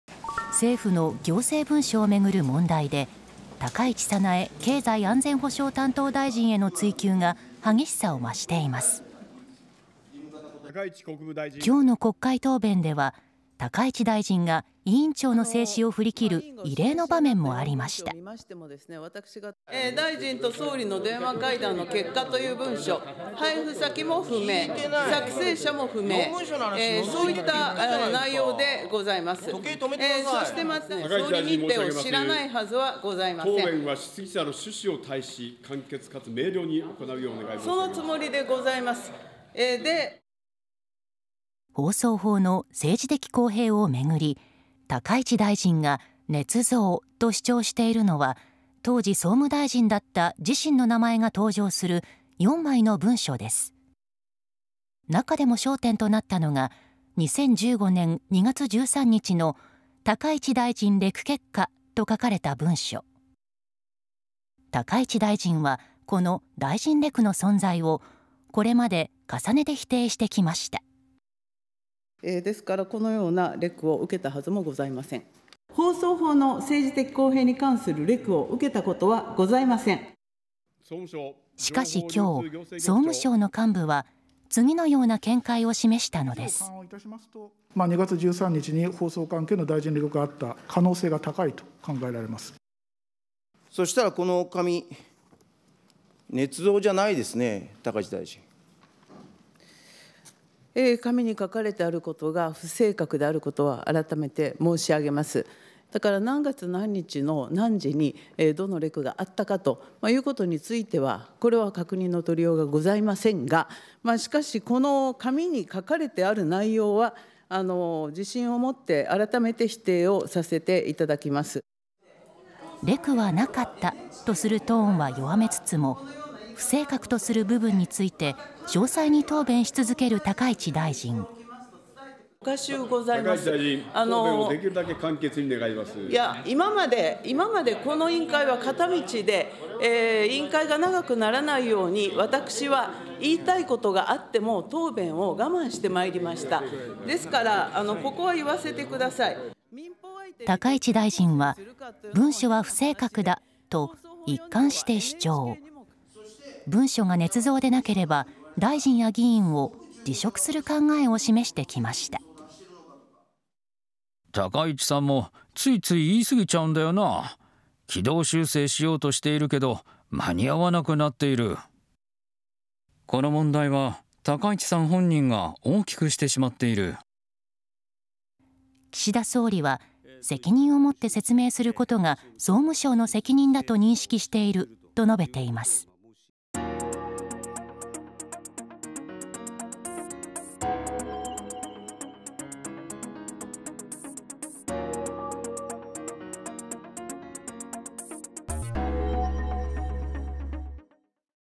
高市経済安保相が“存在を否定”も... 大臣レク「あった可能性高い」 “放送法”めぐり総務省が見解
政府の行政文書をめぐる問題で、高市早苗経済安全保障担当相への追及が、激しさを増している。

13日の国会答弁では、高市経済安保相が、委員長の制止を振り切る、異例の場面もあった。

高市経済安保相「大臣と総理の電話会談の結果という文書、配布先も不明」

議員「聞いてない!」

高市経済安保相「作成者も不明、そういった内容でございます」

議員「時計止めてください!」

委員長「高市大臣に申し上げます。答弁は、簡潔かつ明瞭に行うようお願いします」

高市経済安保相「そのつもりでございます!」

放送法の政治的公平をめぐり、高市経済安保相が“捏造(ねつぞう)”と主張しているのは、当時、総務相だった自身の名前が登場する4枚の文書。

中でも焦点となったのが、2015年2月13日の「高市大臣レク結果」と書かれた文書。

高市経済安保相は、この大臣レクの存在を、これまで重ねて否定してきた。

高市経済安保相「このようなレクを受けたはずもございません」

高市経済安保相「放送法の政治的公平に関するレクを受けたことはない」

しかし13日、総務省の幹部は、次のような見解を示した。

総務省・小笠原情報流通行政局長「2月13日に、放送関係の大臣レクがあった可能性が高いと考えられます」

立憲民主党・福山議員「この紙、捏造じゃないですね、高市大臣」

高市経済安保相「紙に書かれていることが不正確であることは、あらためて申し上げます。何月何日の何時に、どのレクがあったかについては、確認取りようはございませんが、しかし、紙に書かれている内容は、自信を持って、あらためて否定させていただきます」

レクはなかったとするトーンは弱めつつも、不正確とする部分について、詳細に答弁し続ける高市経済安保相。

委員長「高市大臣! 答弁はできるだけ簡潔に!」

高市経済安保相「いや! 今まで! この委員会は片道で、委員会が長くならないように、私は言いたいことがあっても、答弁を我慢してまいりました。ですから、ここは言わせてください!」

高市経済安保相は、「文書は不正確だ」と、一貫して主張。

文書が捏造でなければ、大臣や議員を辞職する考えを示してきた。

閣僚関係者「高市さんも、ついつい言い過ぎちゃうんだよな。軌道修正しようとしているけど、間に合わなくなっている」

自民党中堅議員「この問題は、高市さん本人が大きくしてしまっている」

岸田首相は、「責任を持って説明することが、総務省の責任だと認識している」と述べている。